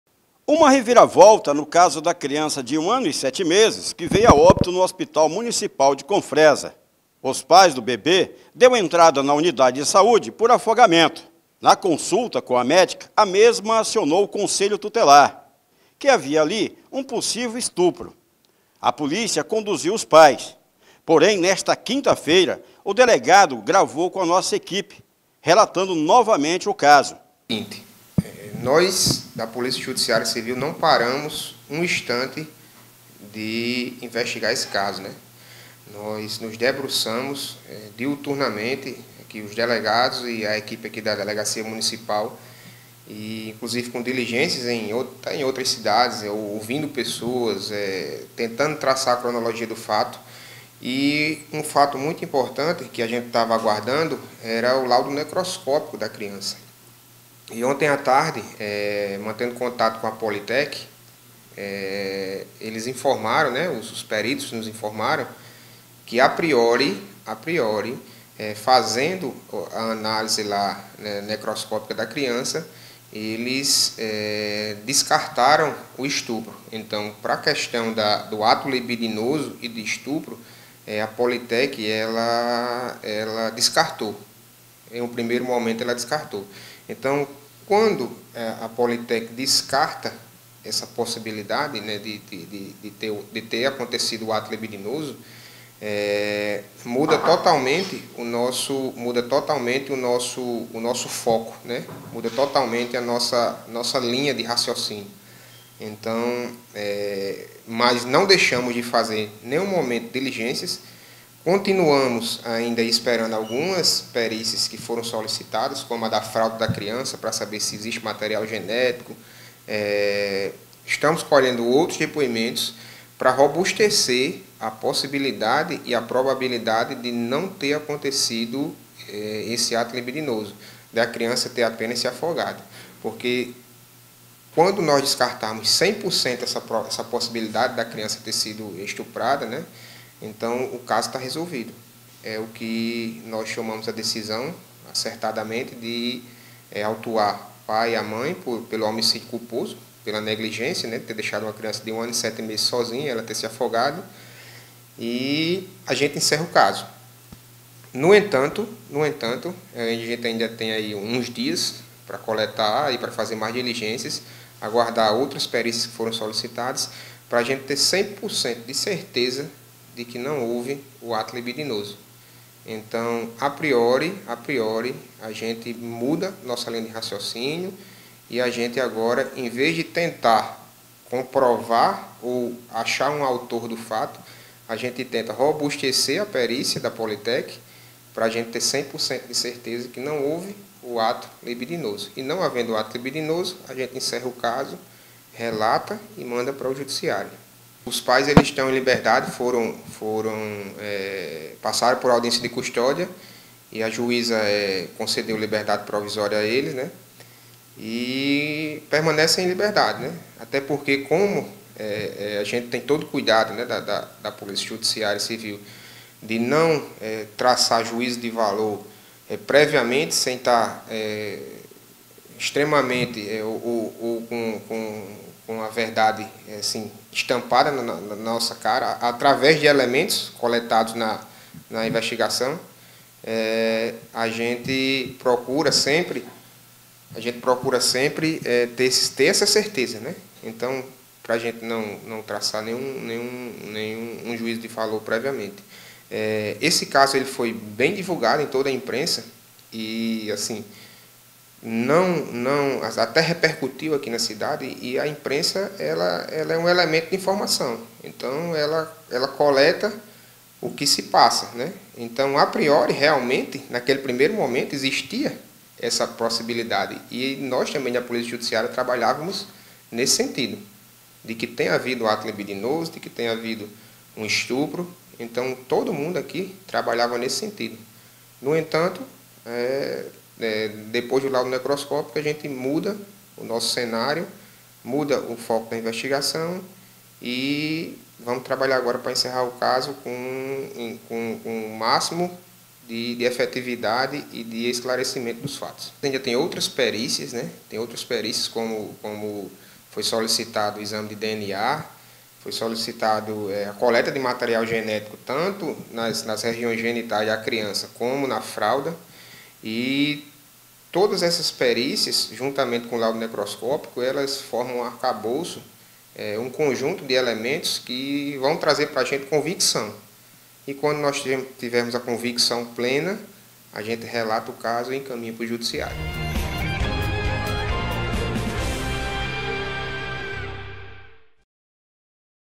REVIRAVOLTA: Autópsia de criança de 1 ano e 7 meses que morreu no Hospital de Confresa
REVIRAVOLTA: Autópsia de criança de 1 ano e 7 meses que morreu no Hospital de Confresa após se afogar descarta estupro, delegado da PJC afirma que investigações continuam